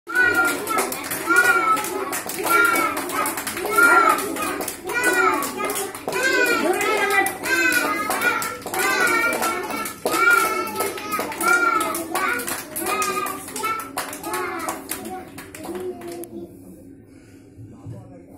Anganwadi school activities /ICDS Patancheru
Encoure the small kid's before joing to pre-school they're are ready to face And lead the world School Anganwadi teacher k.sujatha